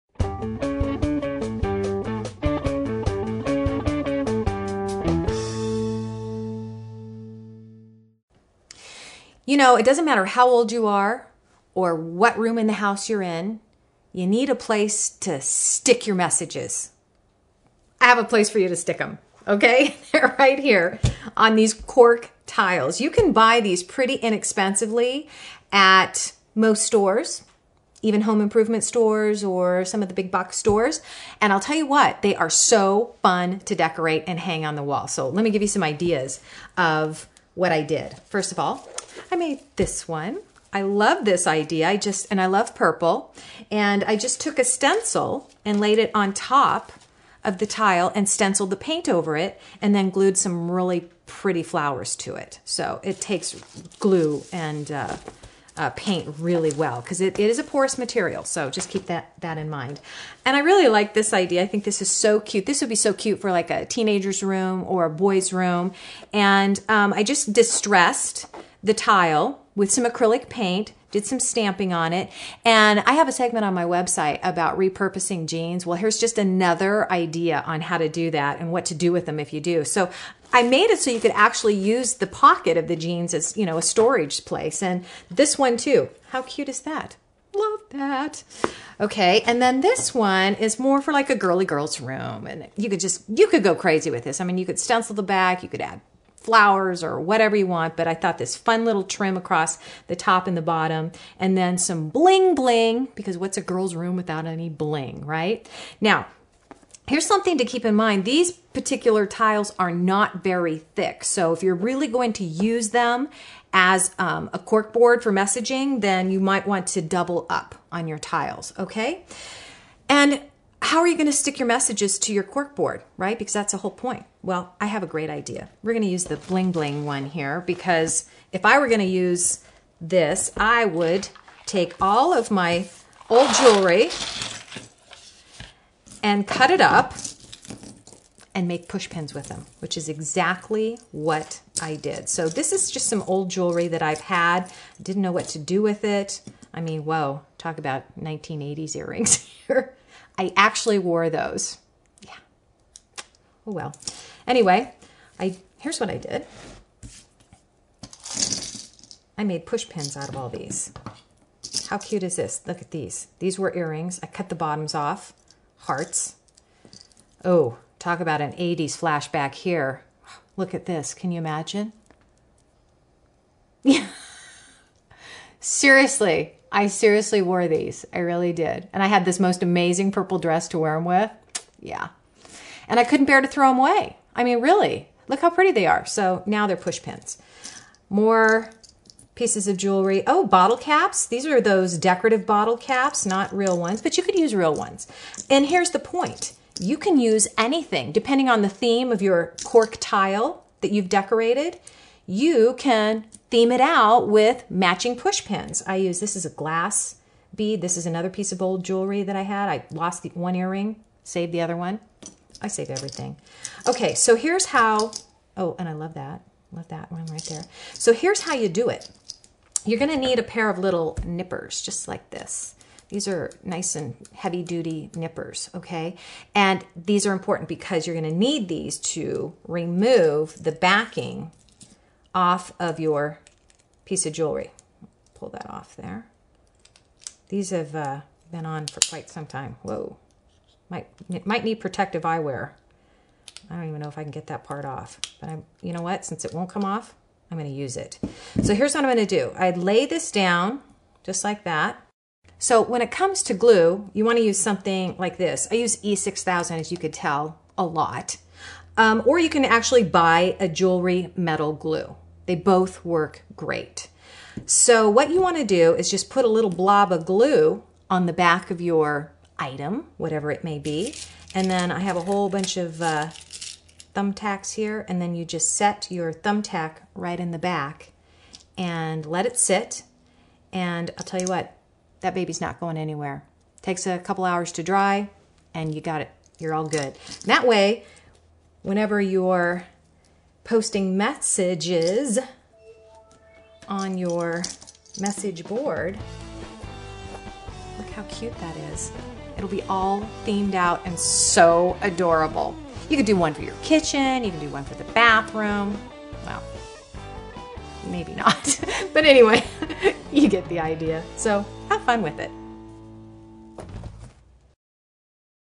Decorative Cork Tiles
This is a really easy and fun way to keep your kids organized while decorating their room at the same time. Be sure to check out how I made some matching push pins!